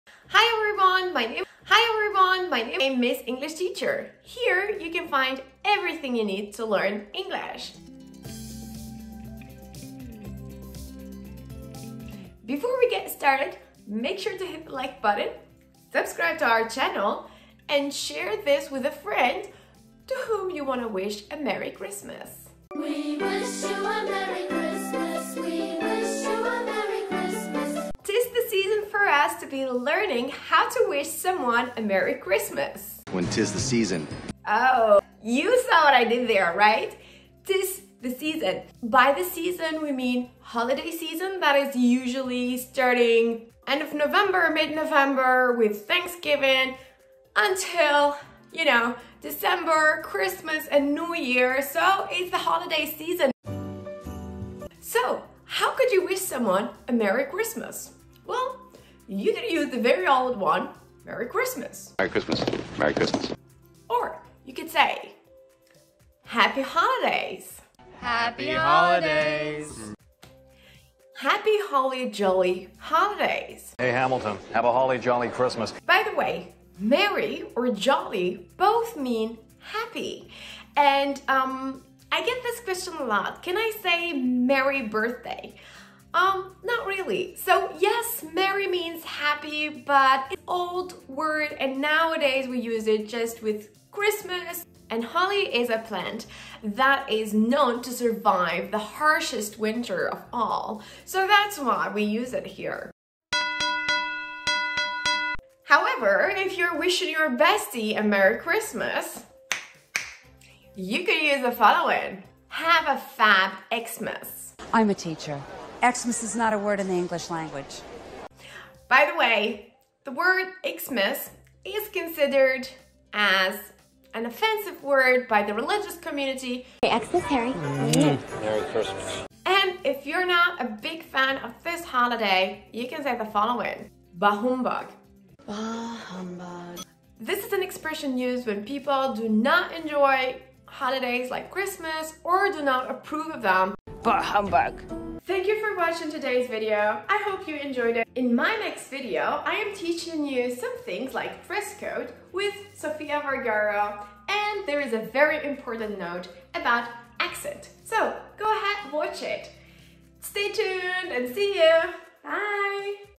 Stop saying "Merry Christmas" - Use these 5 other phrases instead
5 other ways to say "Merry Christmas" - advanced English vocabulary!
In this super short lesson, you will learn other ways to say "Merry Christmas".
If you want to sound original, stop saying "Merry Christmas" and use these 5 other phrases instead. We know you have been waiting for some Christmas vocabulary in English, so, our beautiful teacher Marwa prepared this awesome lesson about "Merry Christmas" greetings in English!
You can learn English AND have fun in our amazing app